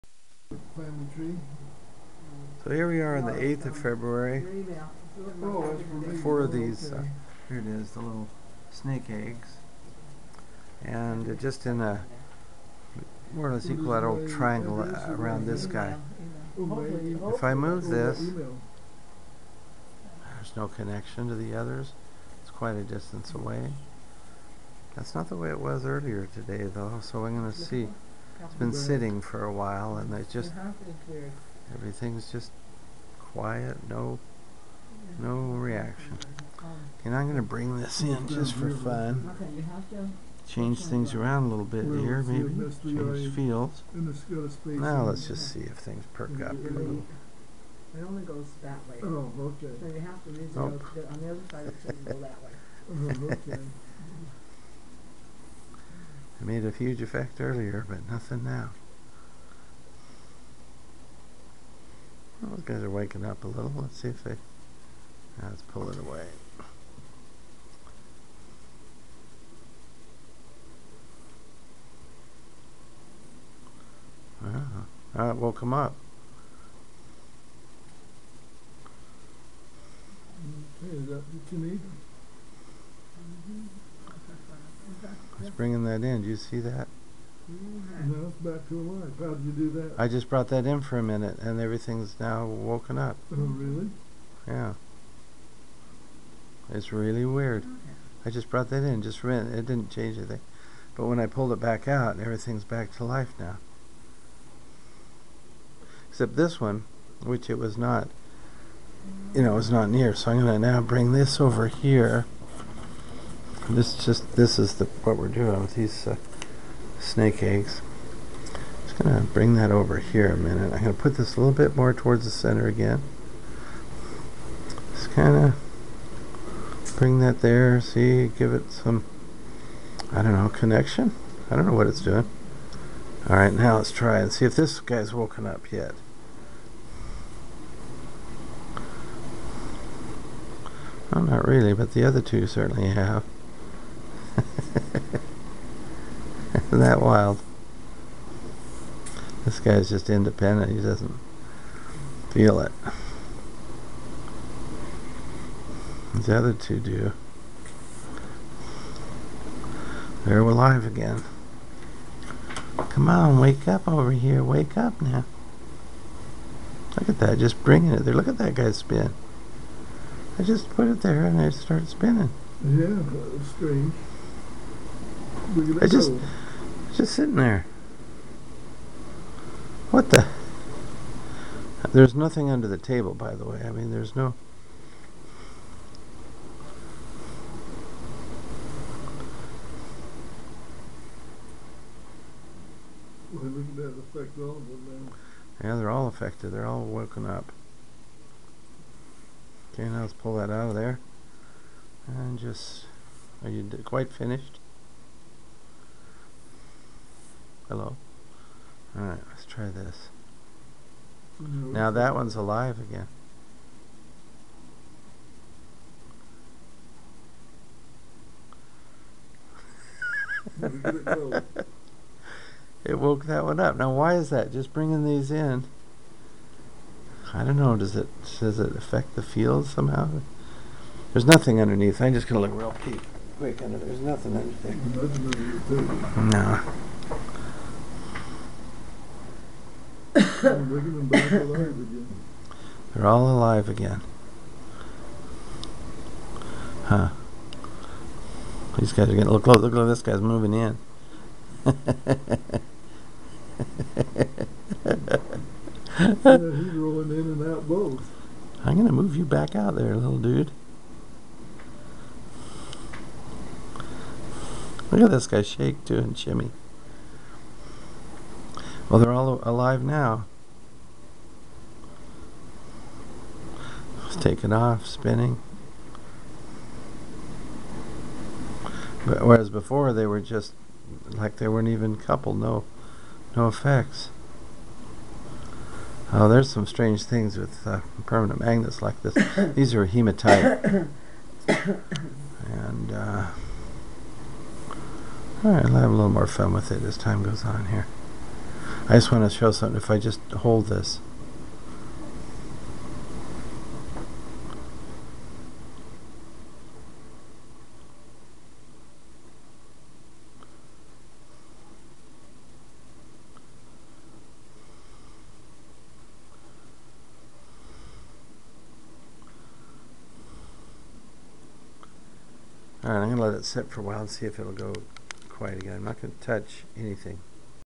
SeeMagnetEggsReCouple
On February 8th, I purchased a bunch of "snake eggs" from Dollar Tree ($1 per pair), which are magnetic hematite in oblong shapes.  I gave some to the grandchildren, and we began playing with them, first in pairs, then threes and finally in fours - as shown in this video.

The experiments are done on an ordinary kitchen table, with family observing. There are no moving magnets in the area - just those you see on top of the table. Three of the magnetic eggs form a triangle; the fourth is placed in the approximate center.

 In this video, they start out "uncoupled", that is, moving the central mag-egg does not cause significant movement (if any) in the other three.  Then I show how I learned to "couple" the outer magnets to the central one, so that when one of the coupled magnets is moved, the others rotate (or sometimes quiver) showing a coupling.  First I couple two of the outer magnet-eggs to the central mag-egg.  Finally, I couple the fourth egg to the other three.

I've done this a number of times.  It is repeatable.
Have fun!  and then lets try to explain this "coupling" behavior.
Could this "coupling" and associated motion have wider applications?